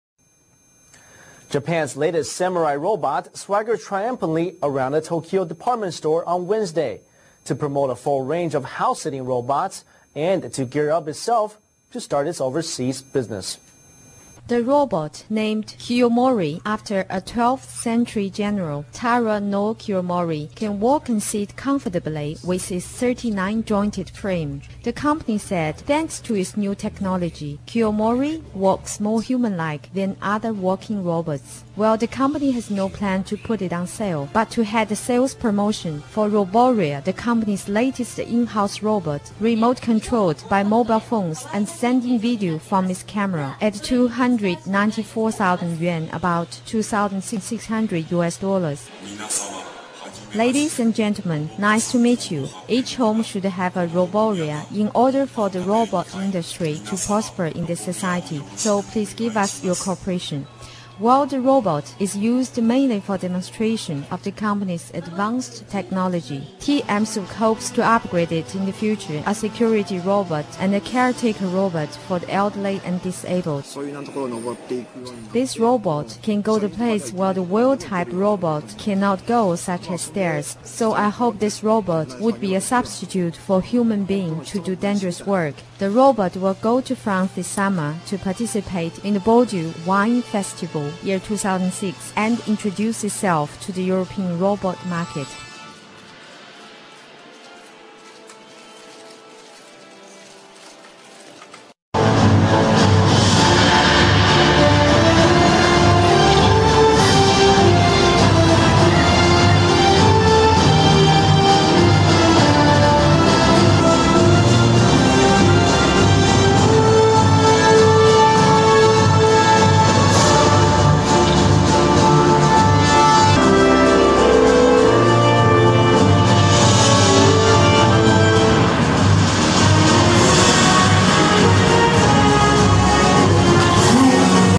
Kiyomori (news report)
This robot was built by TMSUK and Waseda University to promote Japanese robots in late 2005.